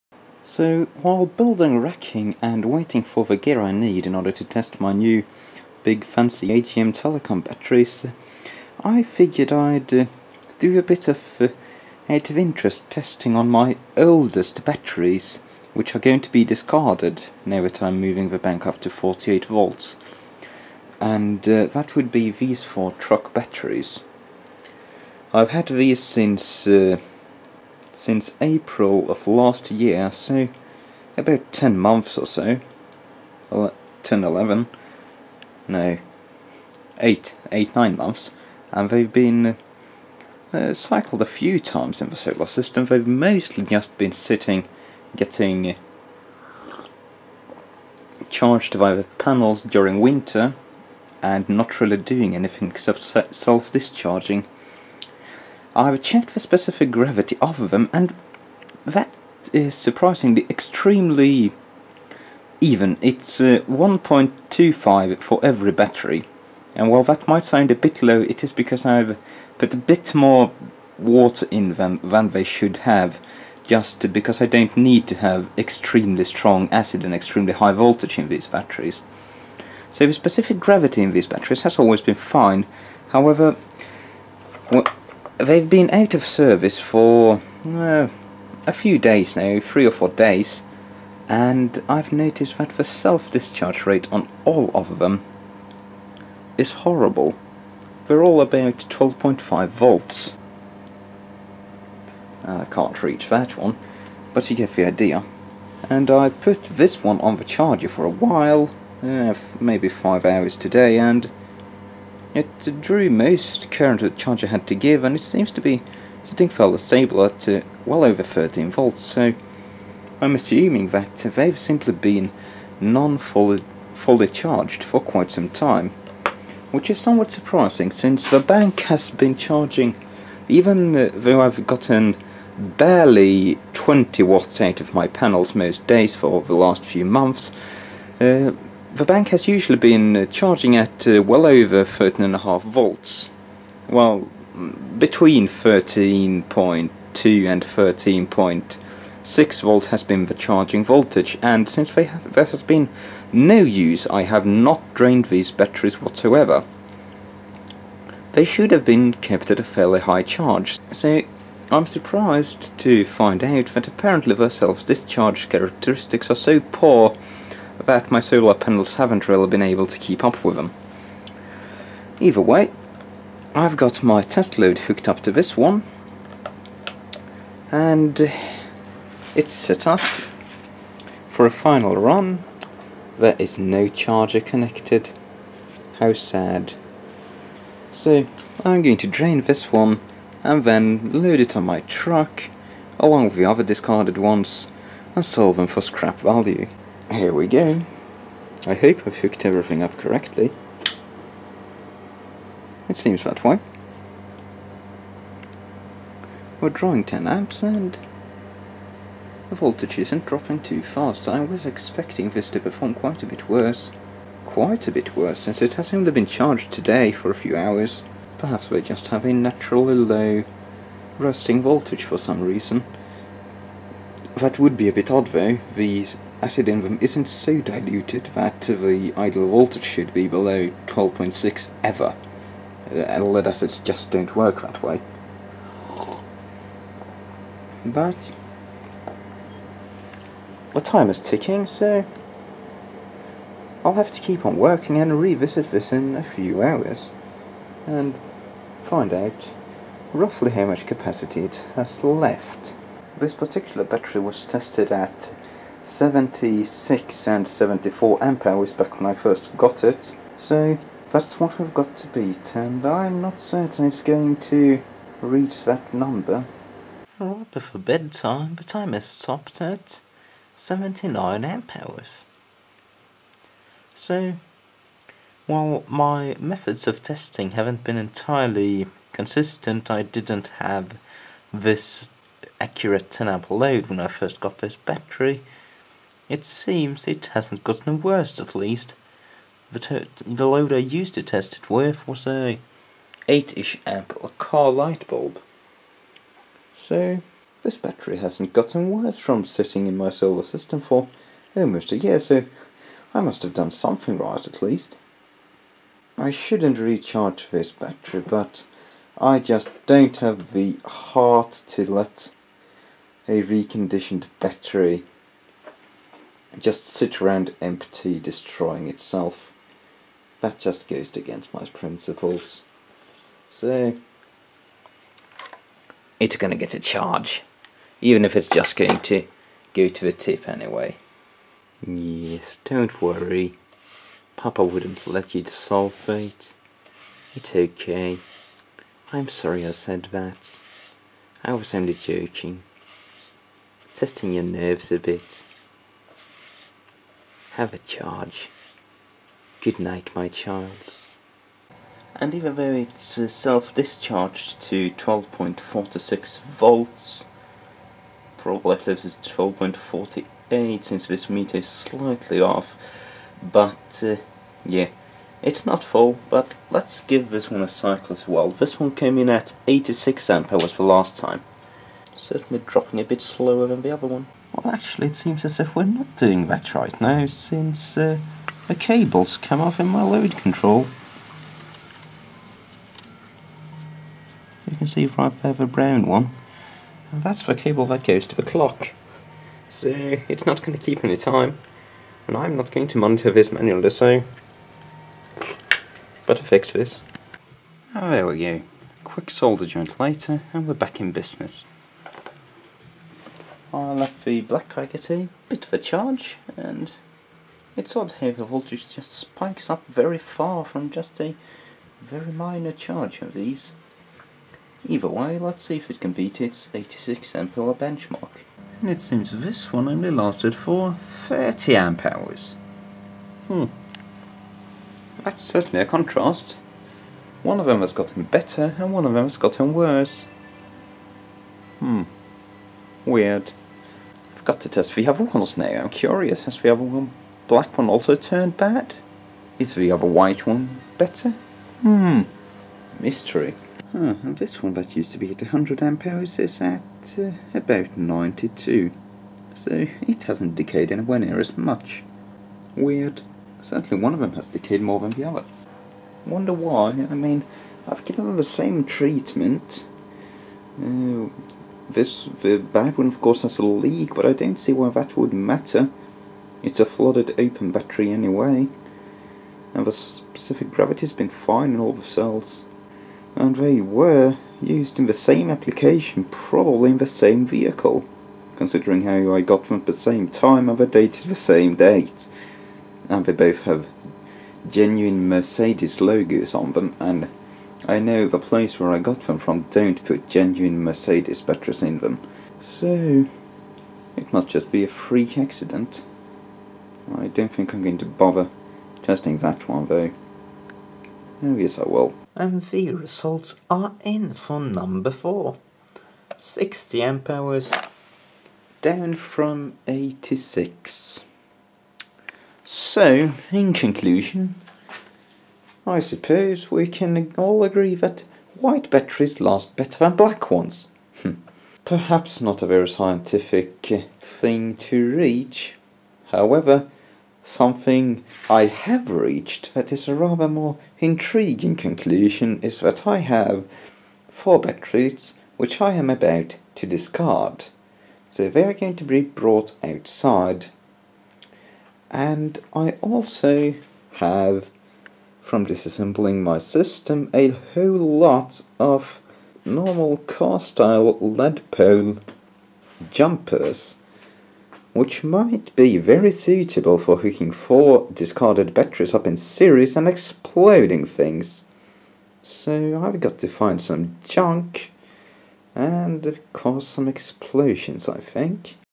Testing the oldest solar batteries before discarding them
Since the 12 V system is going out, I was curious to know how well my reconditioned truck batteries have fared over the past 8 months.

Some of them did surprisingly well, speaking in favour of the viability of actually using truck batteries for cheap solar systems - while others fared not-so-well.

The final abuse of these batteries is coming up very shortly (and it involves burning metal).